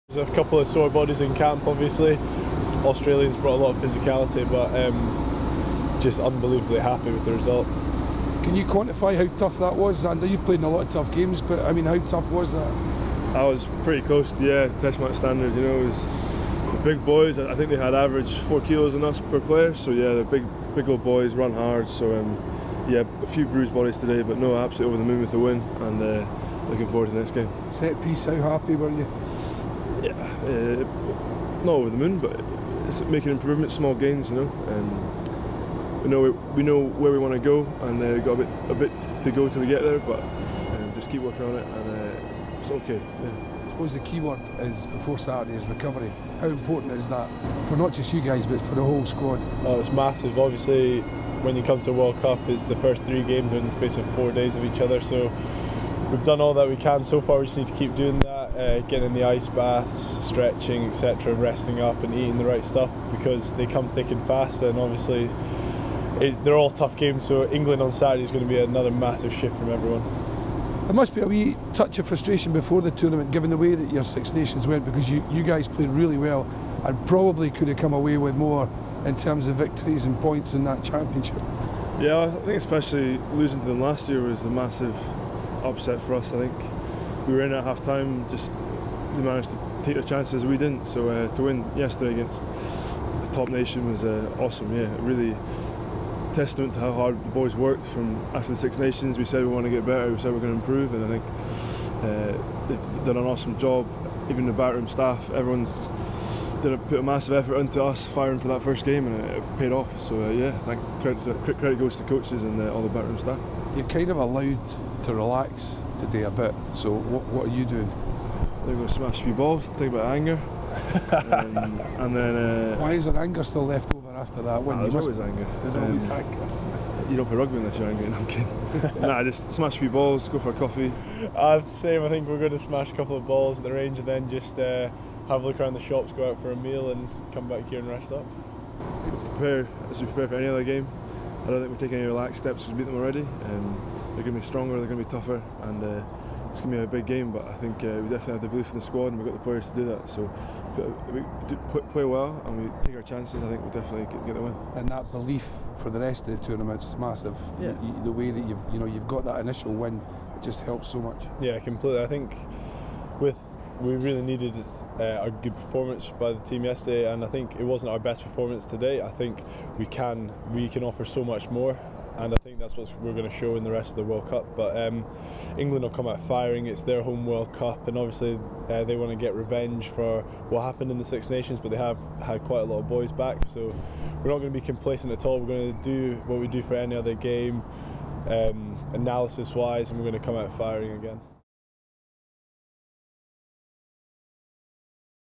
Blair Kinghorn and Zander Fagerson on Scotland's 15-10 win over Australia.
Scotland defeated Australia 15-10 in the World Rugby U20 Championship. Scotland players Blair Kinghorn and Zander Fagerson reflect on that game and look ahead to the match against England.